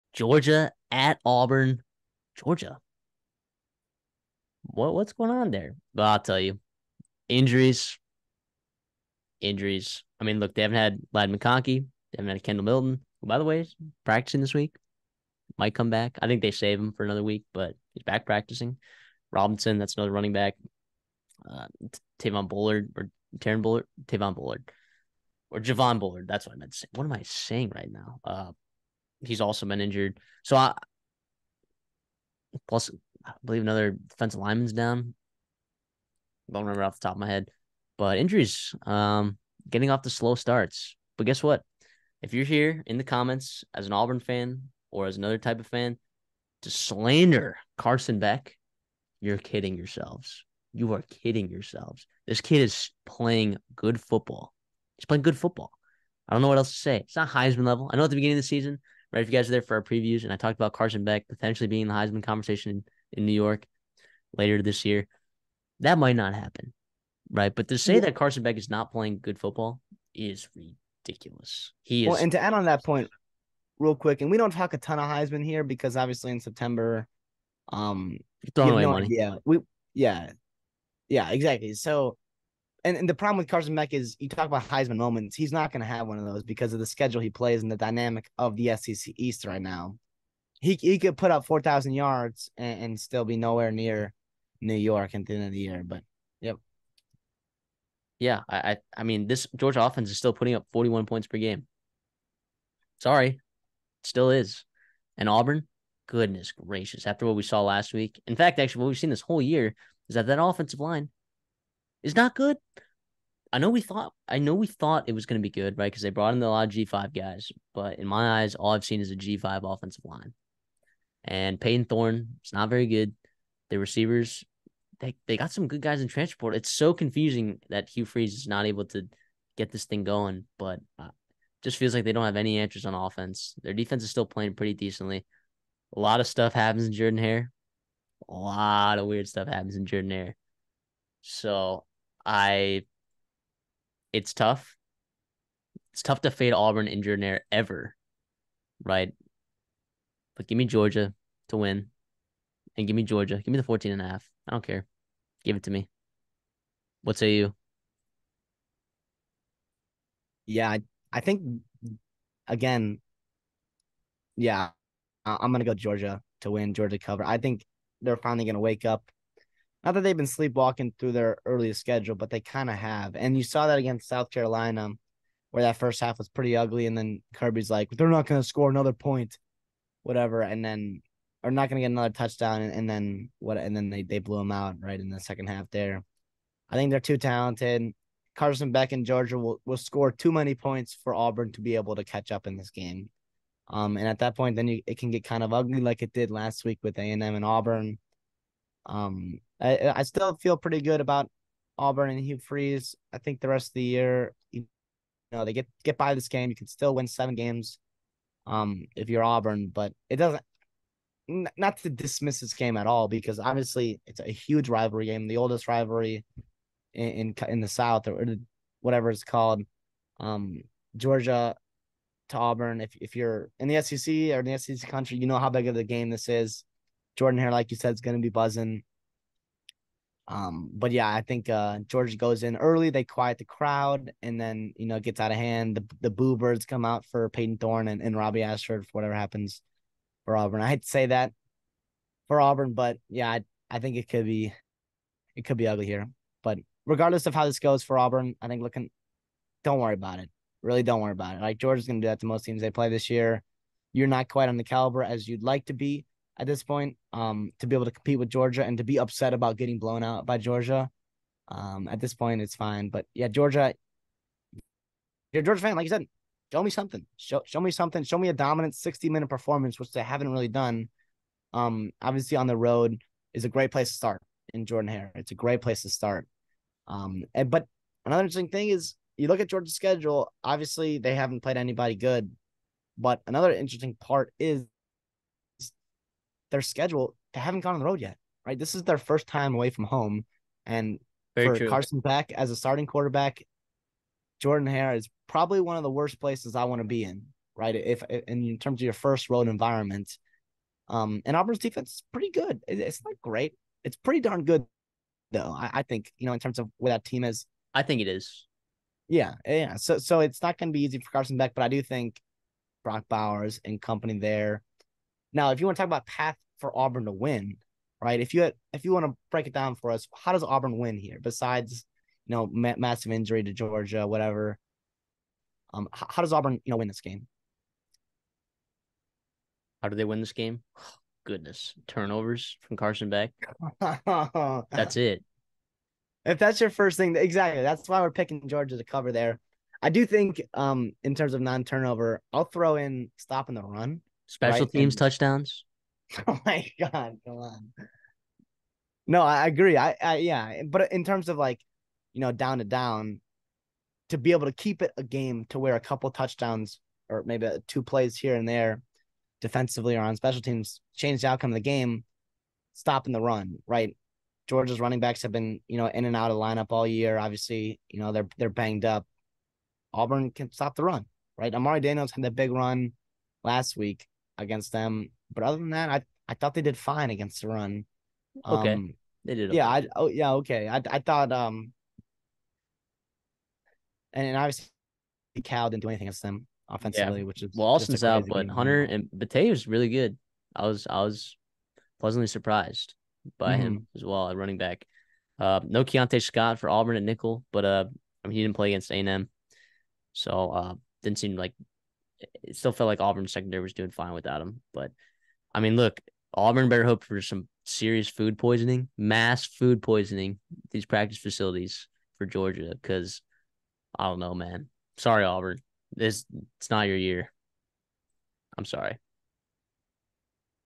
AUBURN VS GEORGIA Week 5 Preview
This is the Redshirt Sophomore Podcast preview, breakdown, and prediction of the Auburn vs Georgia game in Week 5 of 2023.